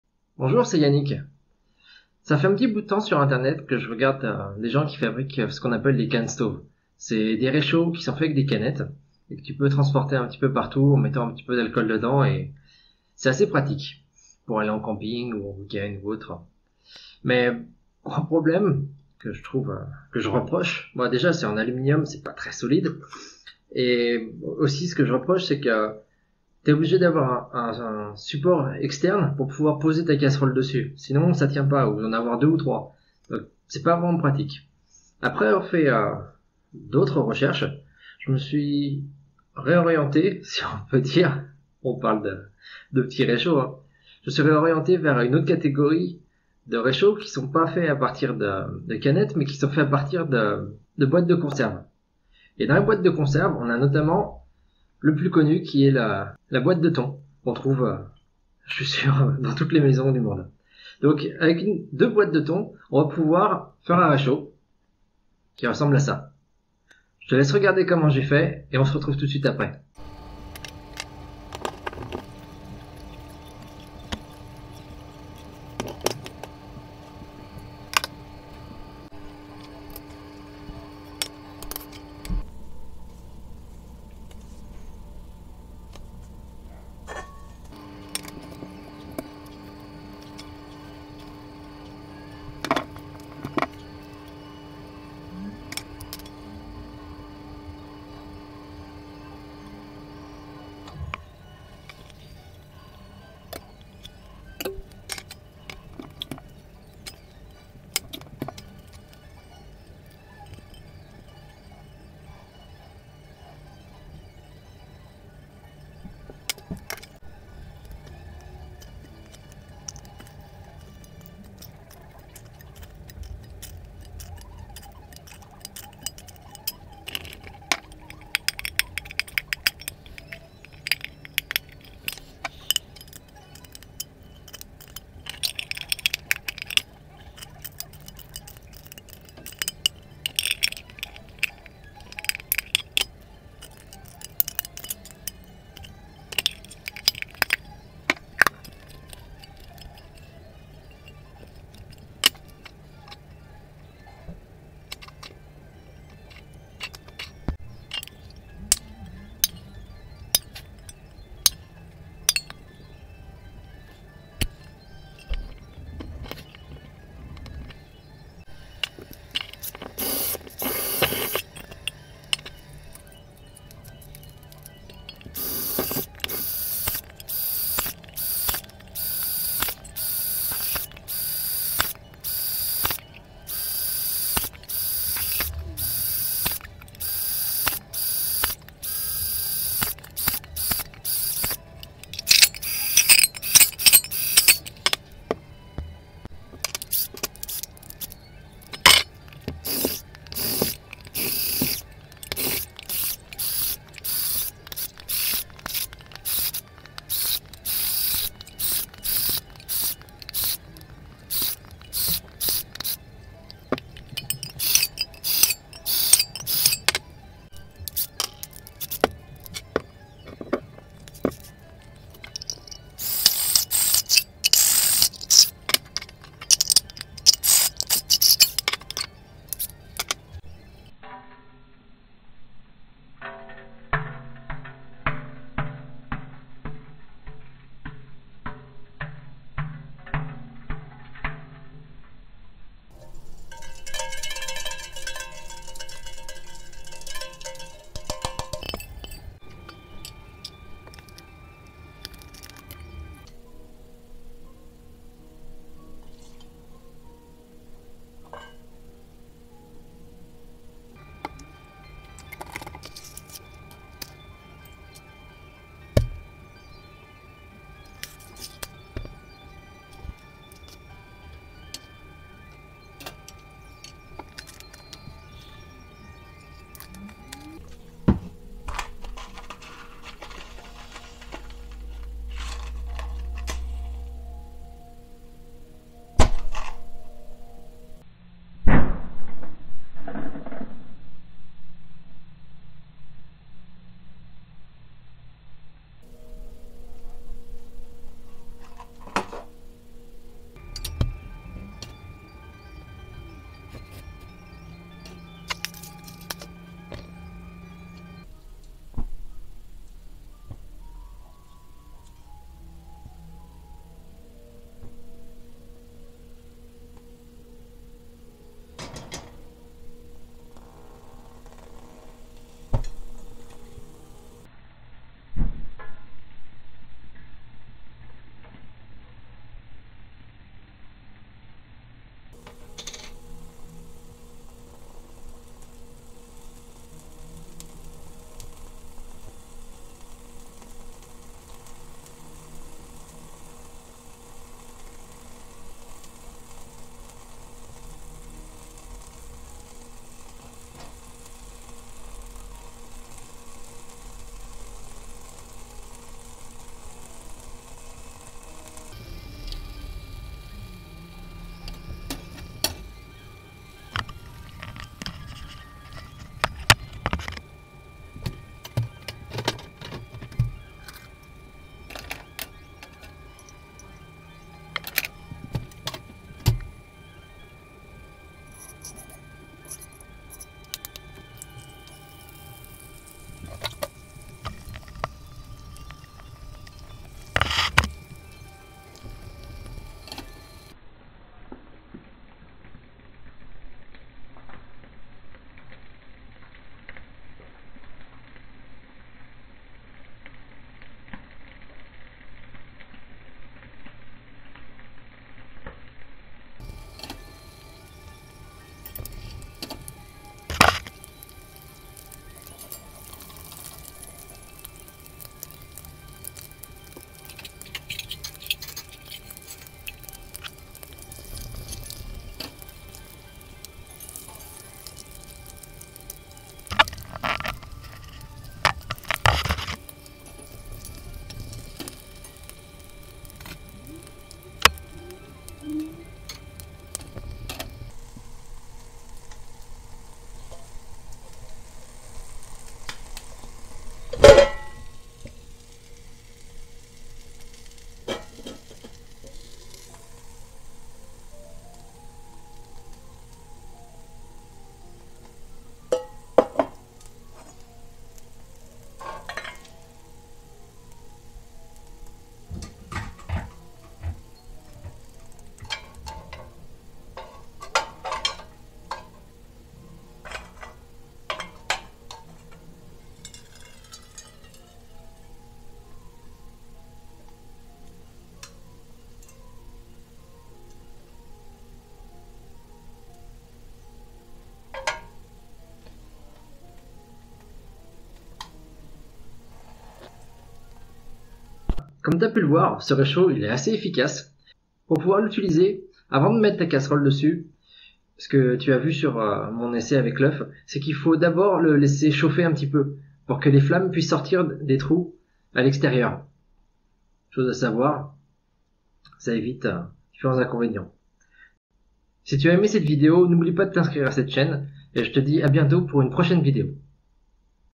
RÉCHAUD ALCOOL 🔥 Peut SAUVER des VIES
Salut les cuisiniers rebelles, dans cette vidéo je vous montre comment transformer 2 boîtes de thon en un réchaud à alcool de plus efficace !

Si vous aimez mes vidéos, vous pouvez m'aider en choisissant l'une des options suivantes:
- en m'achetant un des articles de ma liste de souhaits